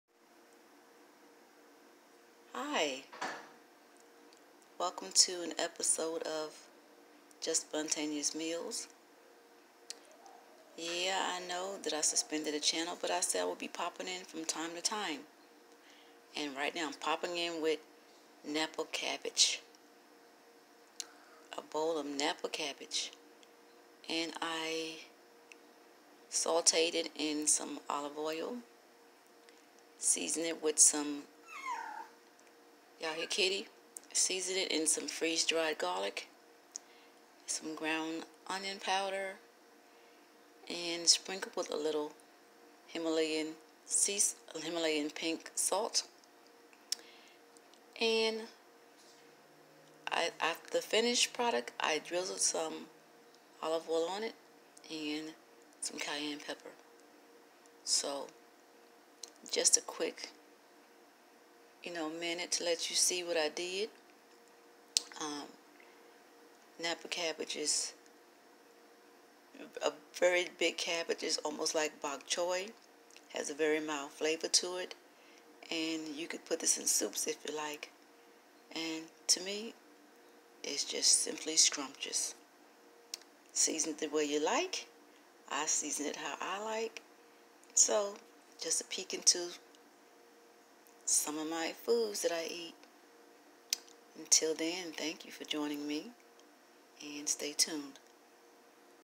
Just Spontaneous Meals: Napa Cabbage 😊
Menu:

Napa Cabbage

Napa Cabbage sauteed in Olive oil, seasoned with Freeze dried Garlic, Onion powder, Himalayan pink salt, Cayenne pepper.

Can be eaten as a meal, or served as a complimentary side dish.

Eat Up! 😊